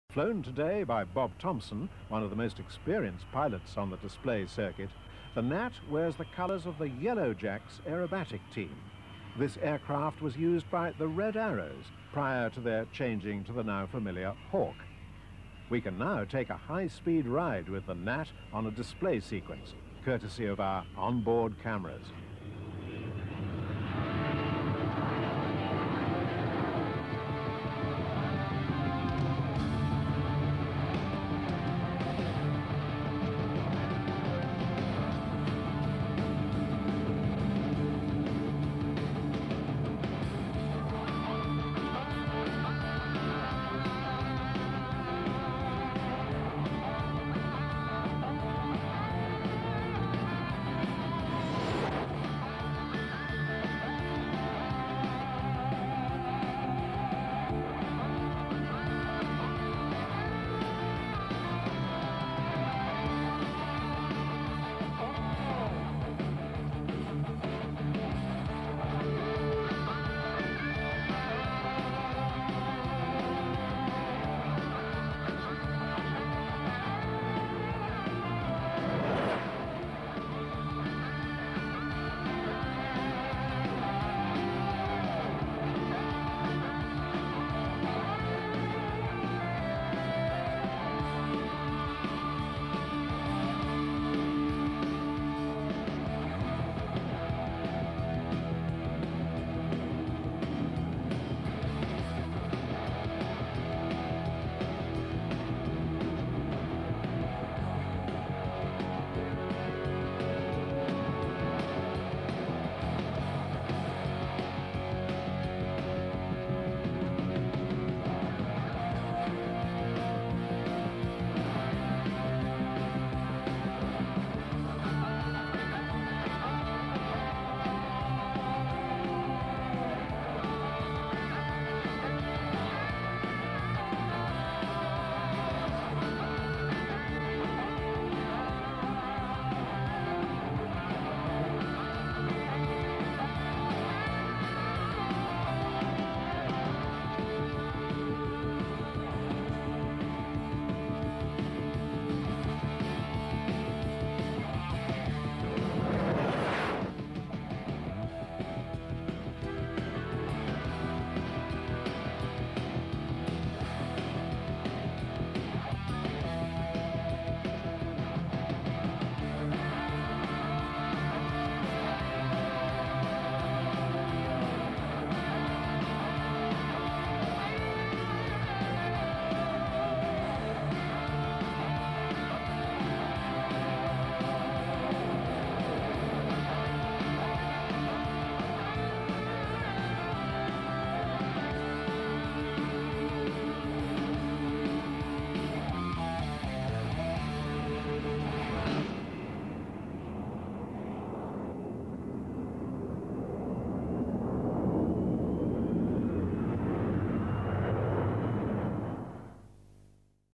Yellowjack Bournemouth Display 24 Aug 1991
Folland Gnat T1 G-MOUR at the Bournemouth Air Display. This video gives you an idea of what it like to fly a low level display in a fast jet. It is a complex aircraft to operate and I had the privaledge of learning to fly it during advanced training  at RAF Valley in 1966 before going onto PR3 Canberras. I renewed my acquaintance with the aircraft in 1991 and flew it on displays in the UK for 9 years without incident. Its been said that flying this aircraft is the best fun you can have with your clothes on.